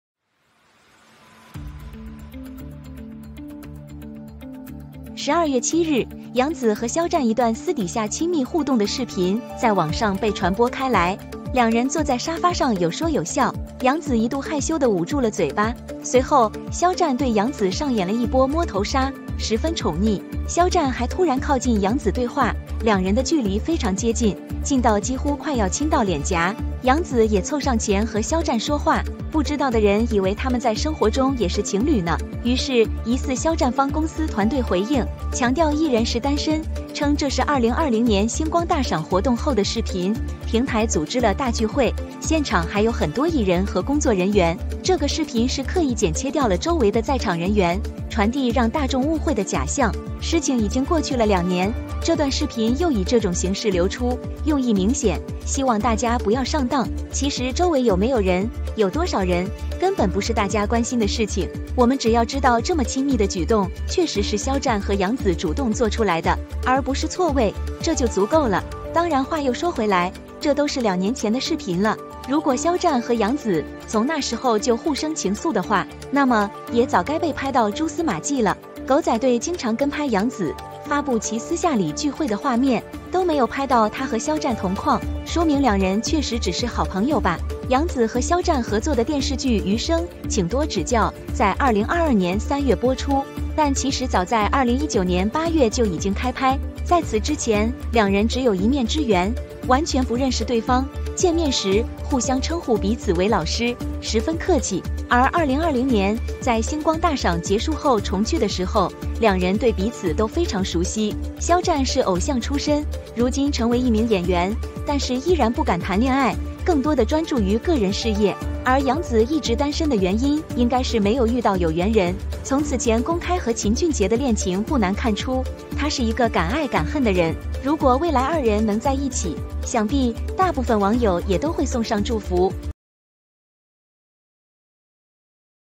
The intimate interaction between Yang Zi and Xiao Zhan was exposed, and the male company refuted the
On December 7, a video of Yang Zi and Xiao Zhao interacting with each other in private went viral on the Internet. The two sat on the sofa talking and laughing, Yang Zi once shyly covered her mouth, and then Xiao Zhan staged a wave of head touch kill on Yang Zi, very doting. Xiao Zhan also suddenly close to Yang Zi conversation, the two are very close to the distance, so close that almost close to kiss the cheek. Yang Zi also came forward to talk to Xiao Zhan, do not know people think they are also a couple in life! The company's team responded by emphasizing that the artist was single, saying that this was the video after the 2020 Starlight Awards event, the platform organized a big party, and there were many artists and staff at the scene, the video was deliberately cut off around the people present, to convey the illusion of public misunderstanding, things have been two years, the video again in this form out, the intention is obvious, I hope you do not fall for it. I hope you will not fall for it. In fact, whether there are people around, how many people, is not a matter of concern, we just need to know that such an intimate act is indeed Xiao and Yang Zizhi initiative to do out, not misplaced, it is enough. Of course, then again, this is 2 years ago video, if Xiao Zhao and Yang Zi from that time on mutual affection, then also should have been shot to the traces. The paparazzi often follow Yang Zi and release the images of her private parties, but they do not capture her and Xiao Zhan in the same frame, which means that the two are really just good friends. The two of them are just a good relationship, because they have no ghosts in their hearts, so they dare to make such intimate actions in public. The two of them had only met each other on one side before that, and they didn't know each other at all, and they called each other "teacher" when they met, which was very polite. And when they reunited at the end of the Starlight Awards in 2020, they were very familiar with each other. The two of them are chatting more and more. The two of them were also awarded the King and Queen of Weibo Night in 2021, so their popularity is evident. This year in March, "the rest of the life please teach more" finally opened, the rest of the couple sweet sugar, many viewers into the play too deep, hope that the two can fake the real thing, the reality is also together, the TV series during the broadcast, the two often interact, but after the broadcast, there is no joint marketing over. Xiao Zhi is an idol, now an actor, but still afraid to fall in love, more focused on personal career, and Yang Zi has been single because it should not meet the right person, from the previous public and Qin Junjie's romance is not difficult to see, she is a dare to love and hate people. If the two can be together in the future, I think most netizens will also send their blessings.